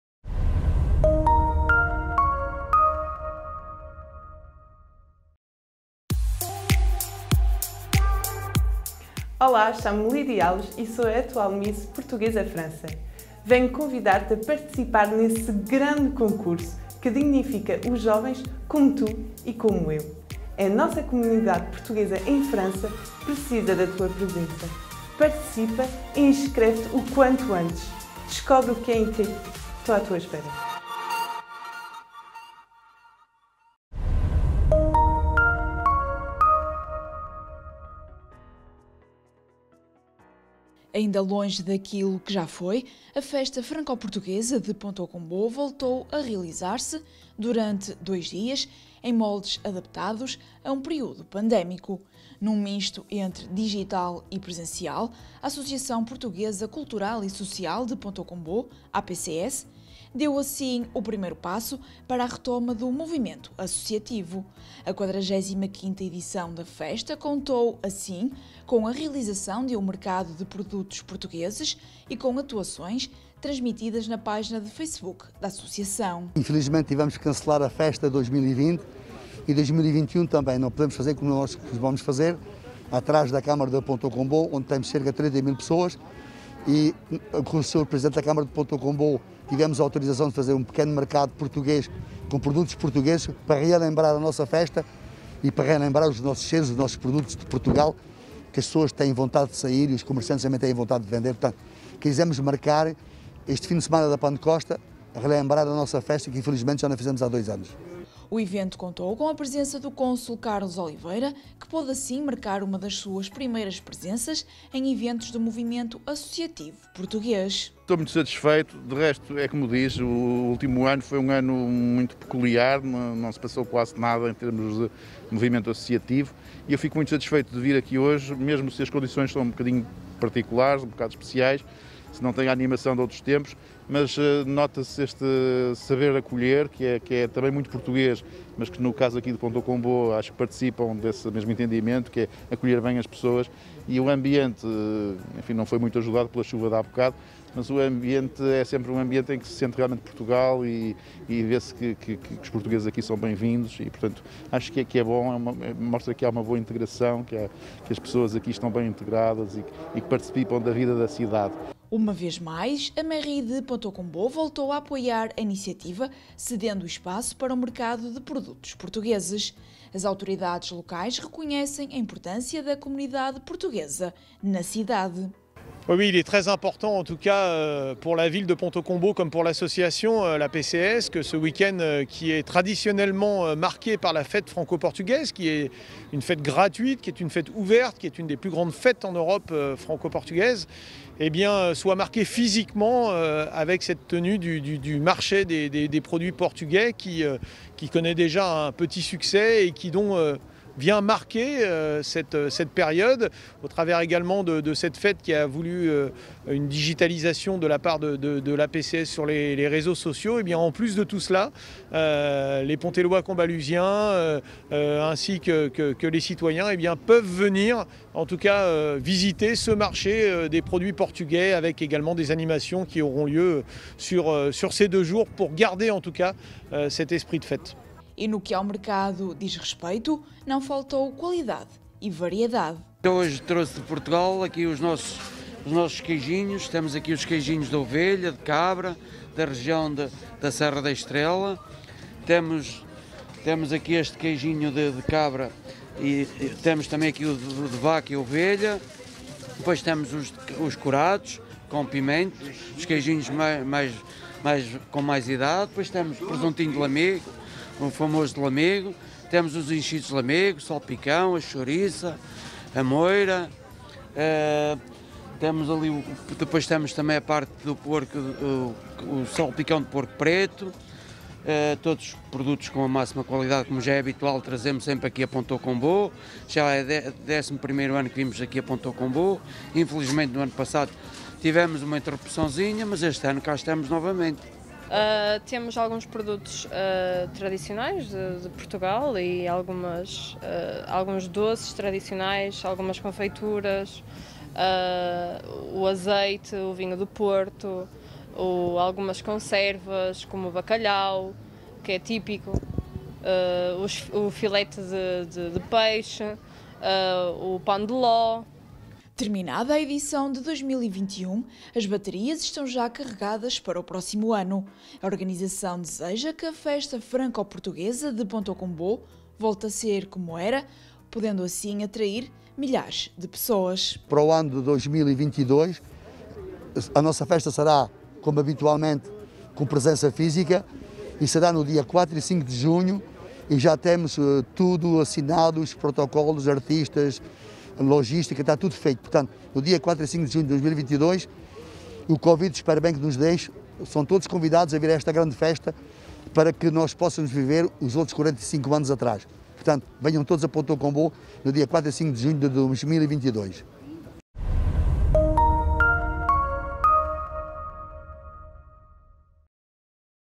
45ª Festa Franco Portuguesa de Pontault-Combault assinalada com mercado de produtos portugueses
Ainda longe daquilo que já foi, a Festa Franco-Portuguesa de Pontault-Combault voltou a realizar-se, durante dois dias, em moldes adaptados a um período pandémico. Num misto entre digital e presencial, a Associação Portuguesa Cultural e Social de Pontault-Combault, APCS, deu assim o primeiro passo para a retoma do movimento associativo. A 45ª edição da festa contou, assim, com a realização de um mercado de produtos portugueses e com atuações transmitidas na página de facebook da associação. O evento contou com a presença do Cônsul Carlos Oliveira, que pôde assim marcar uma das suas primeiras presenças em eventos do movimento associativo português. Uma vez mais, a Mairie de Pontault-Combault voltou a apoiar a iniciativa, cedendo o espaço para o mercado de produtos portugueses. As autoridades locais reconhecem a importância da comunidade portuguesa na cidade.  E no que ao mercado diz respeito, não faltou qualidade e variedade. Terminada a edição de 2021, as baterias estão já carregadas para o próximo ano. A organização deseja que a Festa Franco-Portuguesa de Pontault-Combault volte a ser como era, podendo atrair milhares de pessoas.